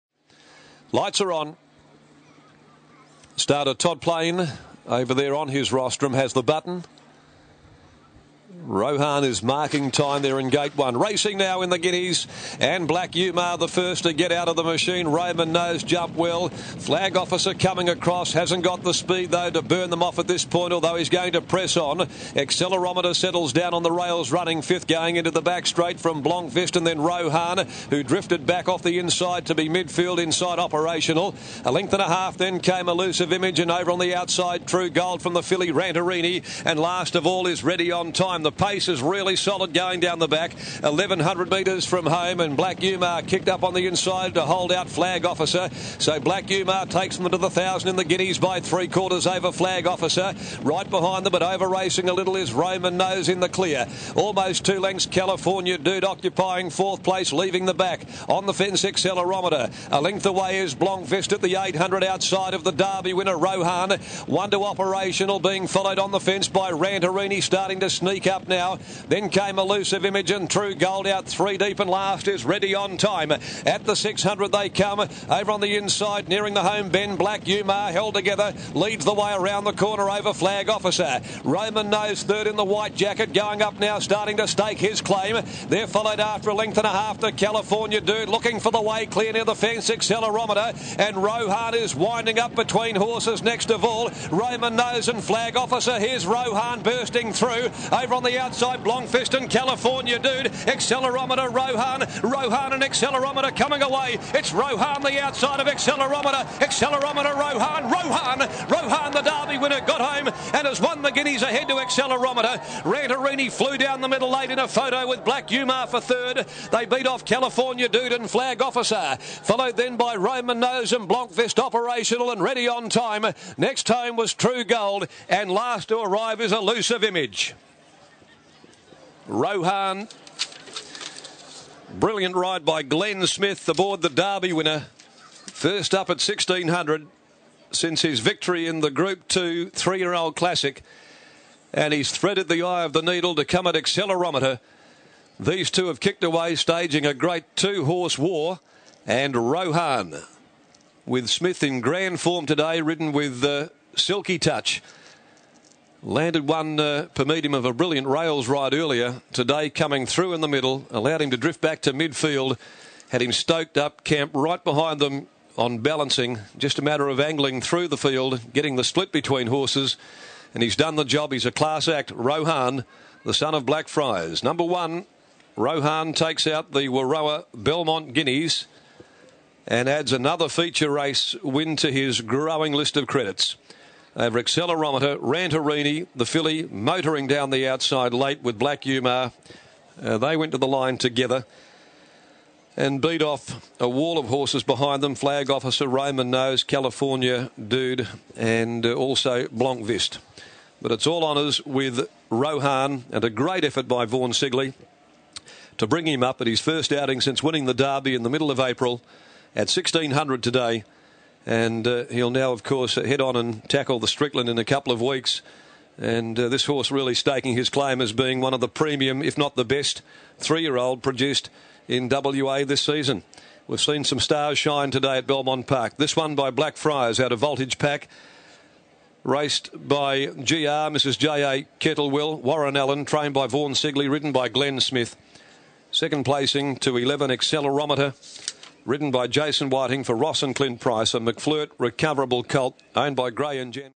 2012 WAROA Belmont Guineas
Won by Rohan - Trained by Vaughn Sigley - ridden by Glenn Smith